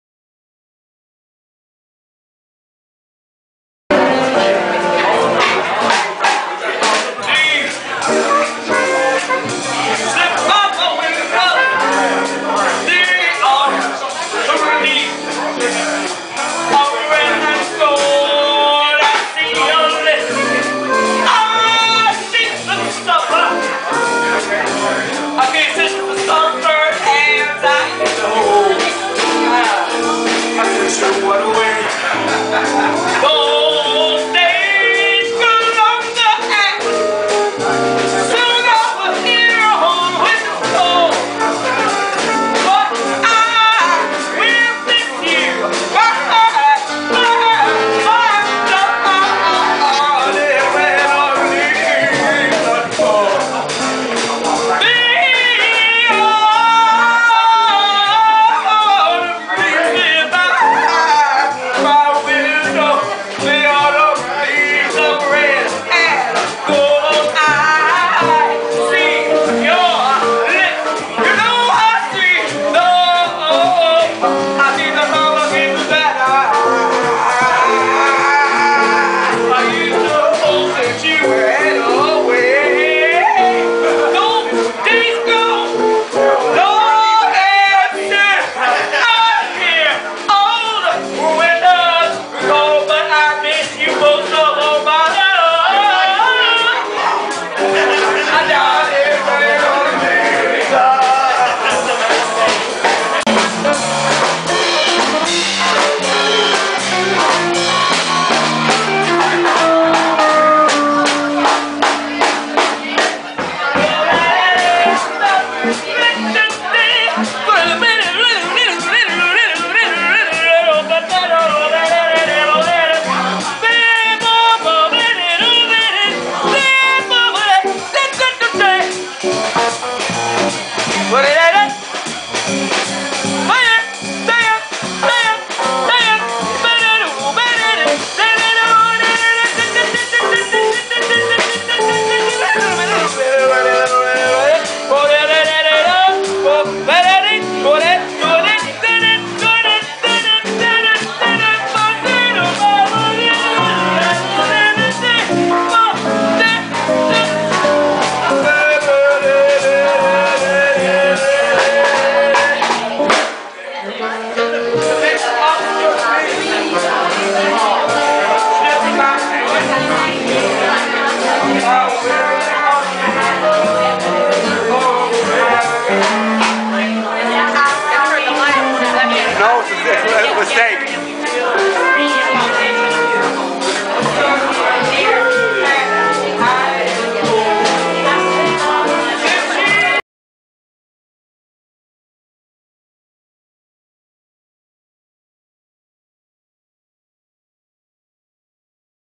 Gwen Sampe singing with Hoodoo
Gwen Sampe and Hoodoo at the Brothers Paris Winter Gala